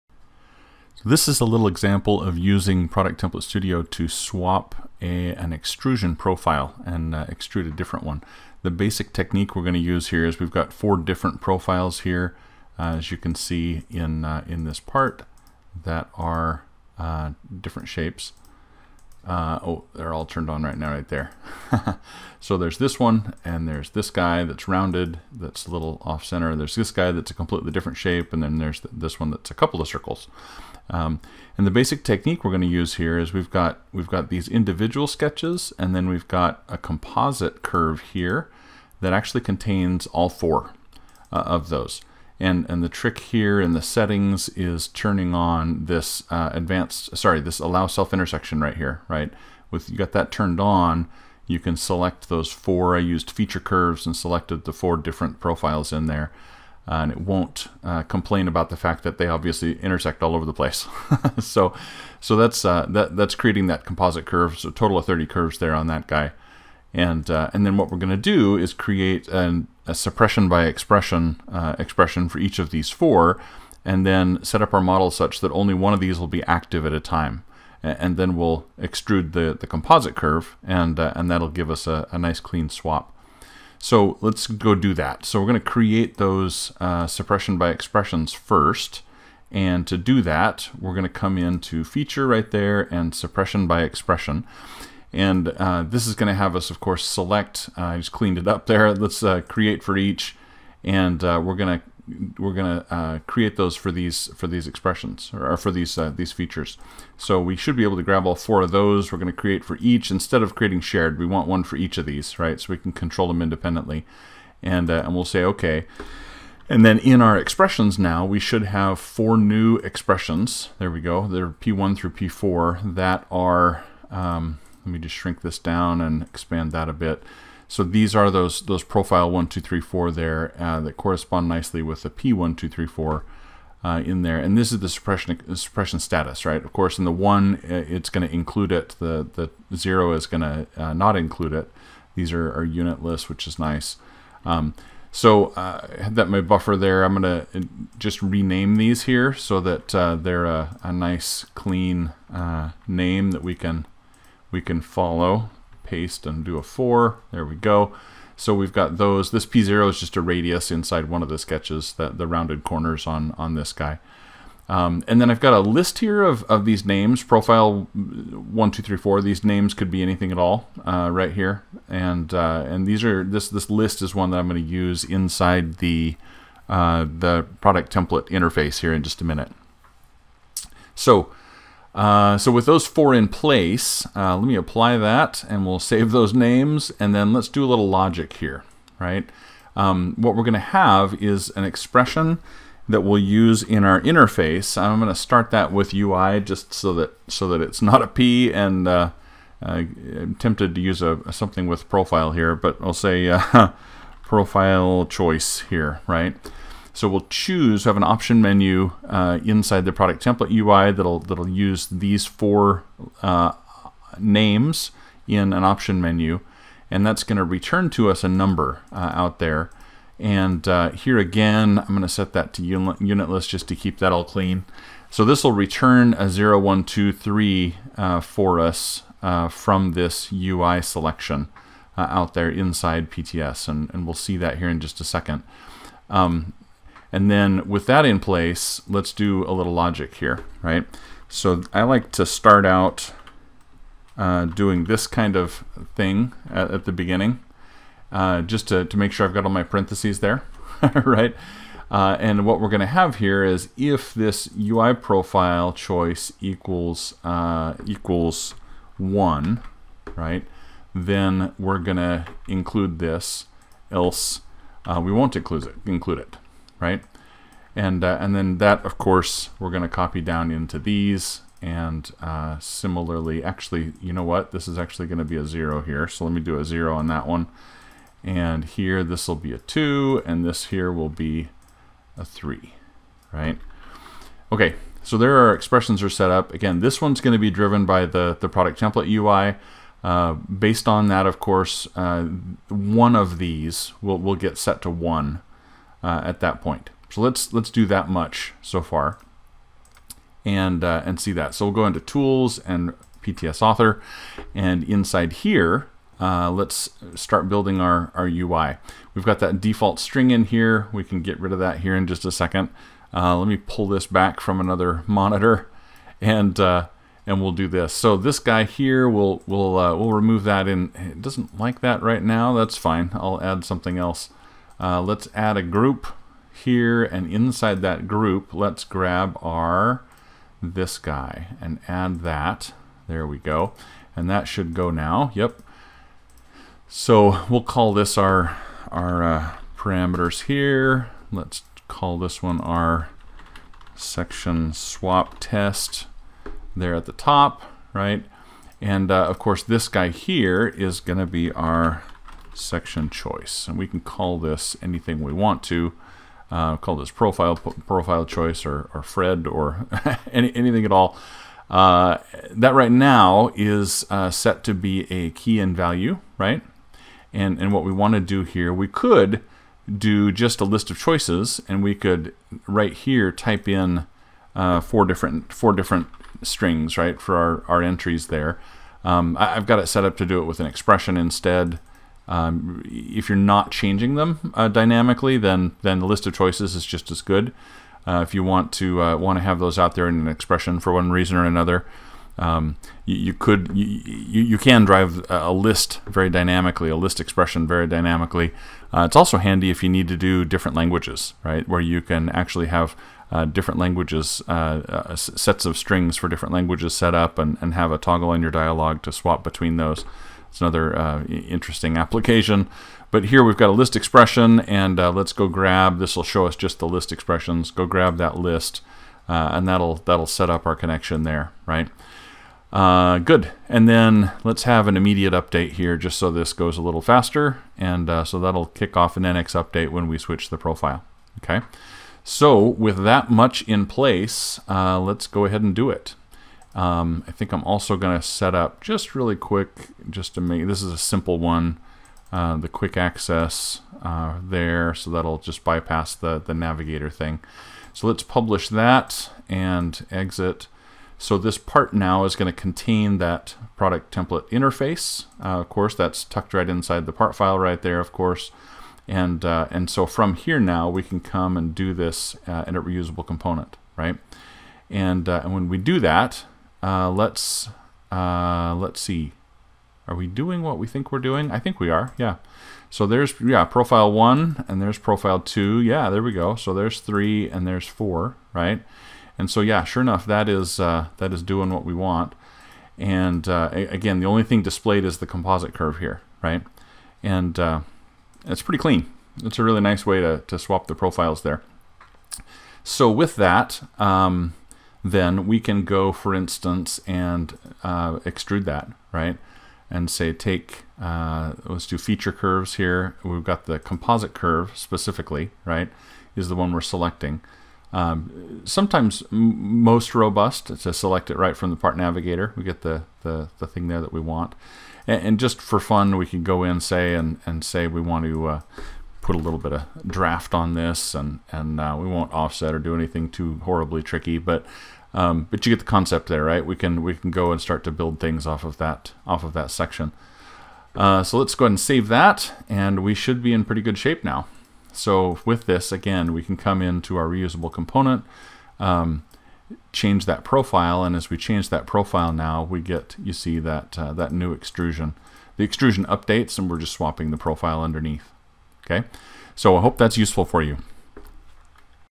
NX Product Template Studio: Swapping Input Sections [NX 1980]
I've had a few questions about using an option menu inside a Product Template interface to swap between sections for an extrude or revolve or sweep feature.  This video goes through the process of building the model and UI to swap between four optional input sections.